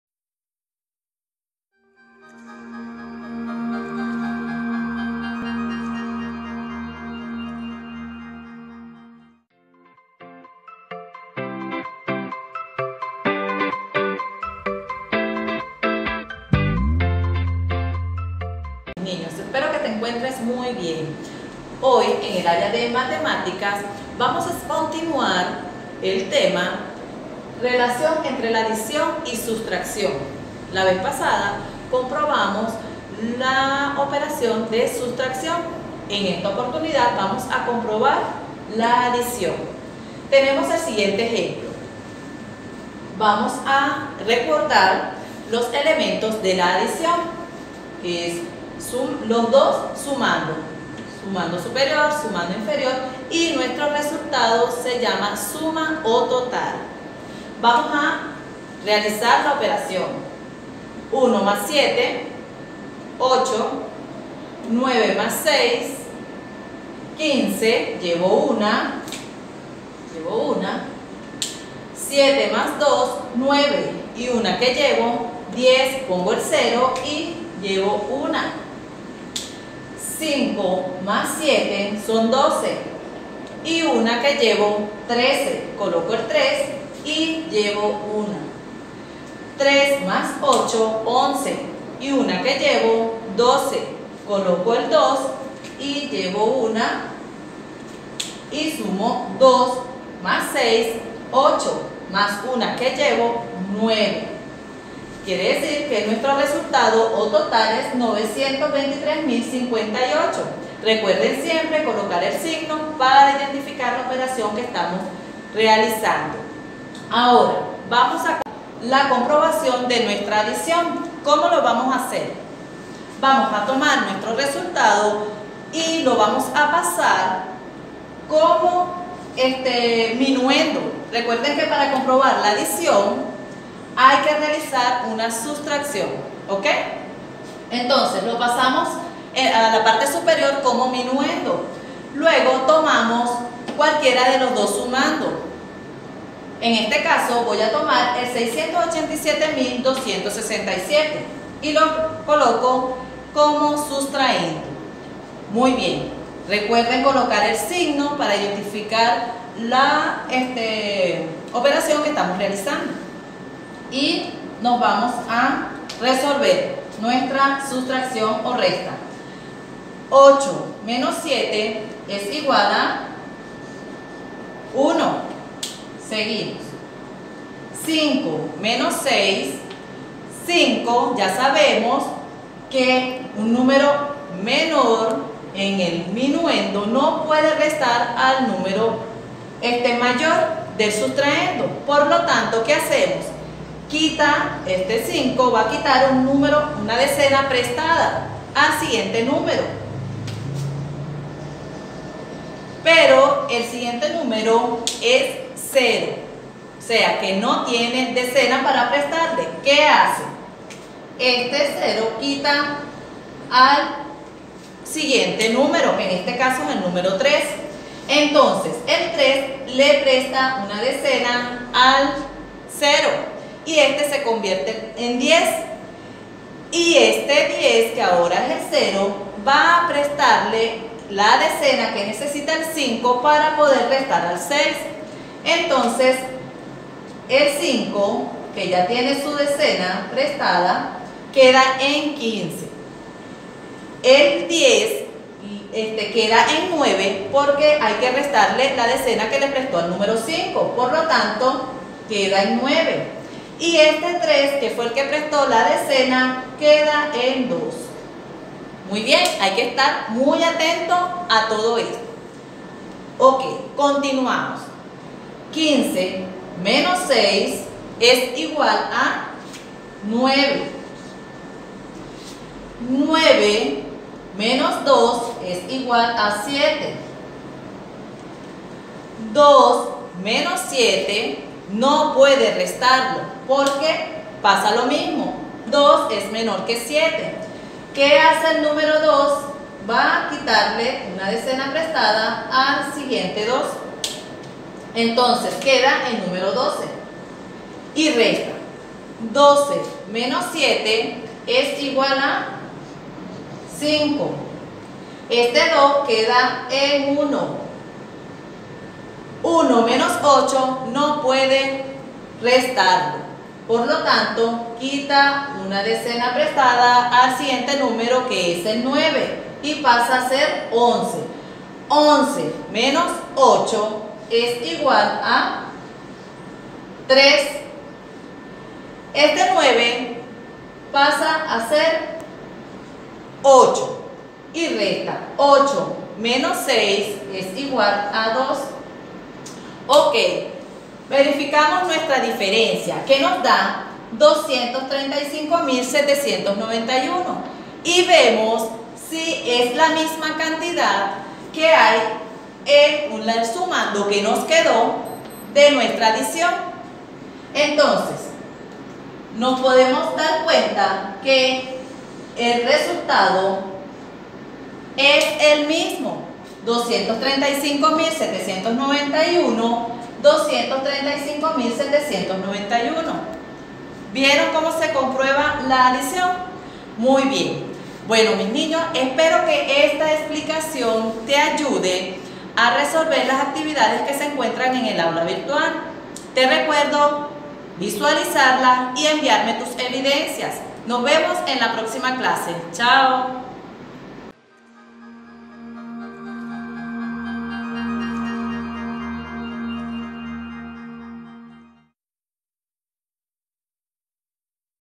Relación entre la adición y sustracción. 3er Grado A Comprobación de la Adición o Suma.
Docente Dayana Acosta